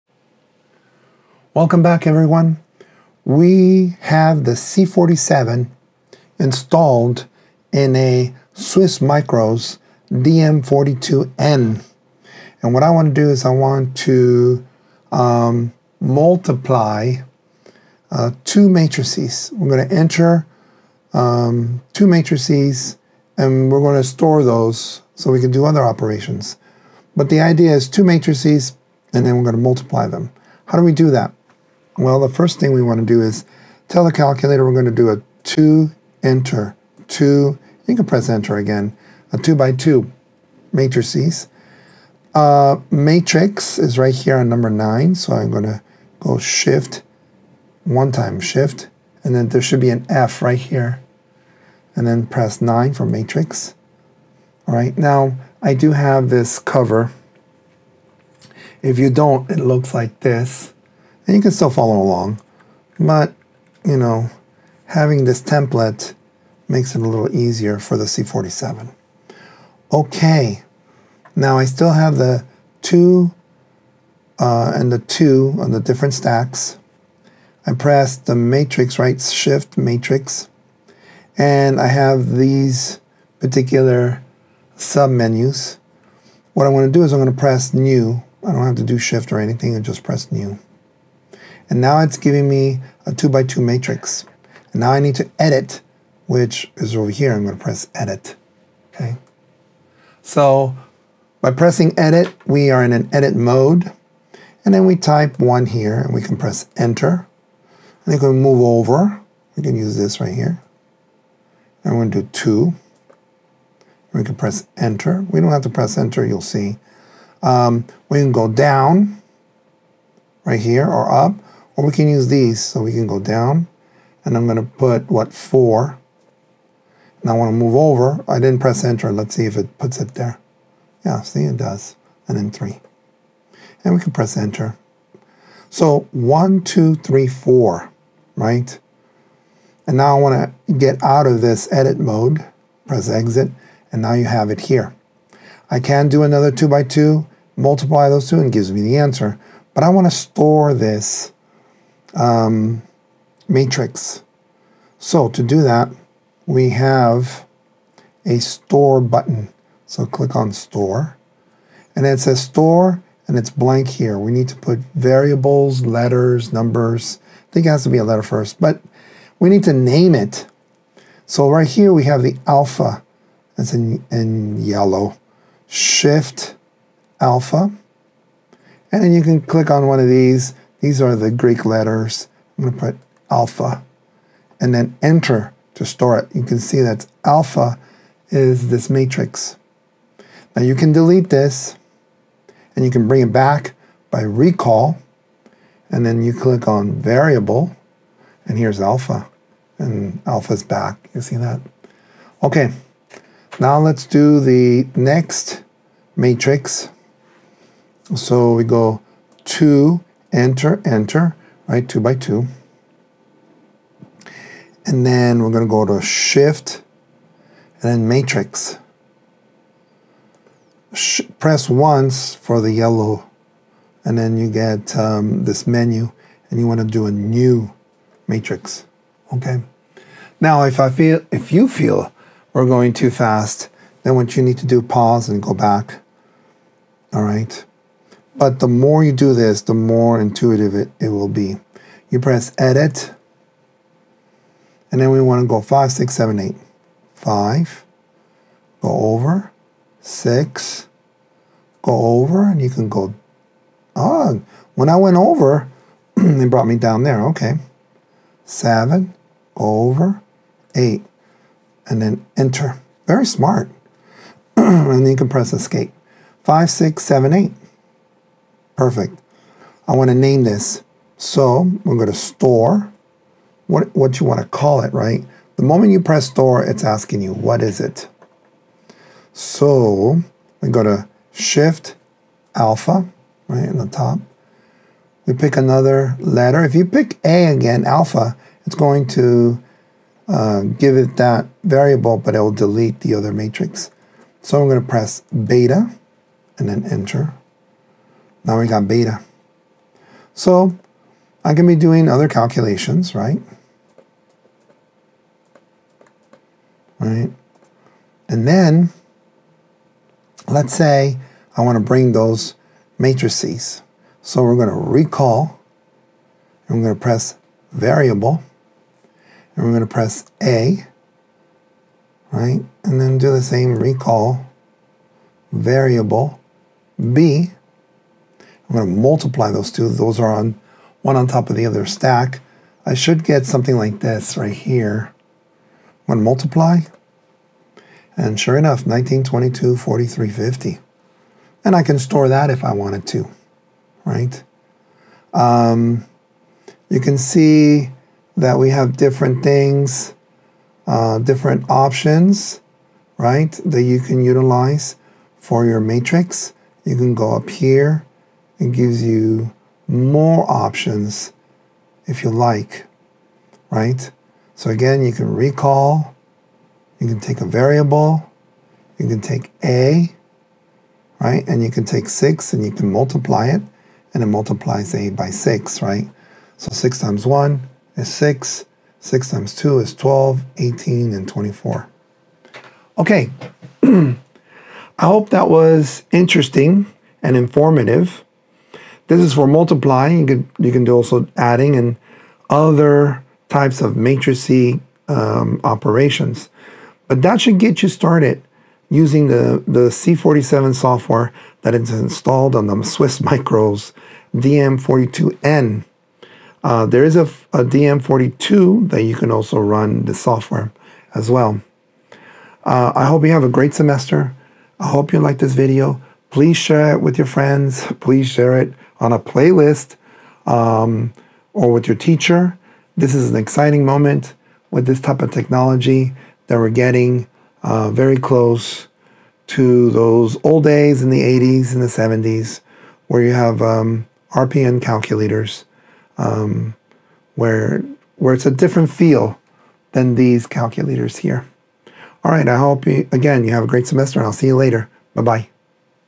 C47 Calculator - Matrices (SwissMicros DM42n)
In this video, I walk through how to use the C47 Calculator running on the SwissMicros DM42n to work with matrices. I demonstrate how to create matrices, store them in memory, and then apply arithmetic operations step by step. The tutorial shows both the calculator’s interface and the logic behind matrix handling, making it a practical guide for anyone exploring linear algebra on the C47 platform.